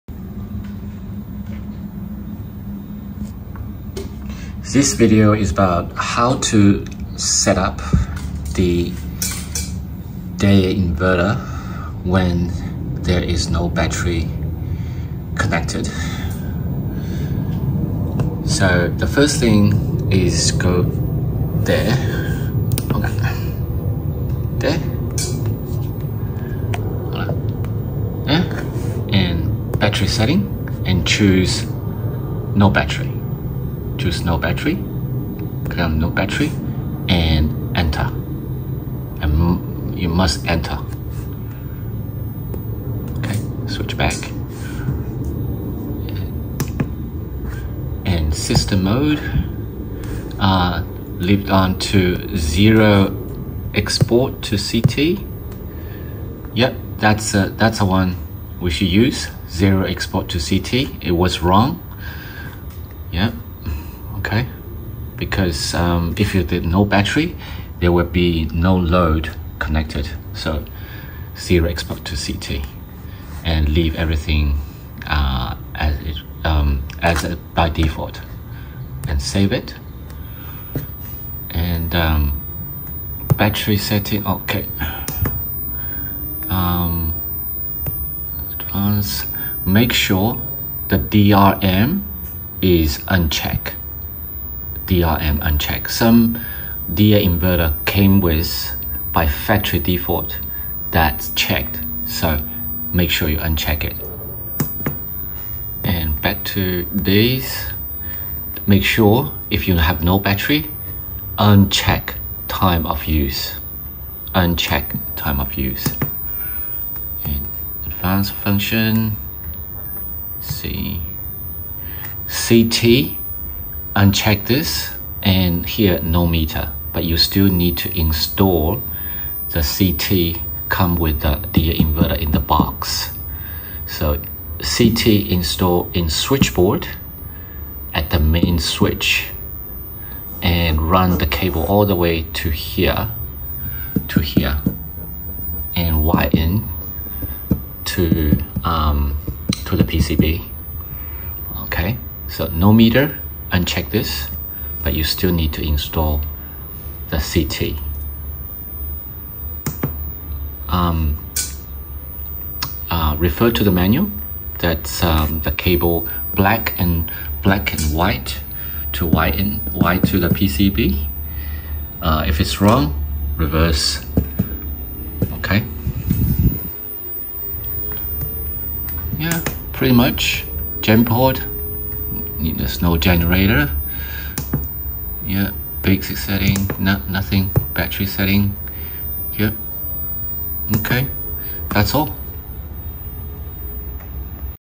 HOW TO: Deye hybrid inverter settings for no storage batteries connected
Deye hybrid inverter settings for no storage batteries connected. It works as a regular grid-tie inverter.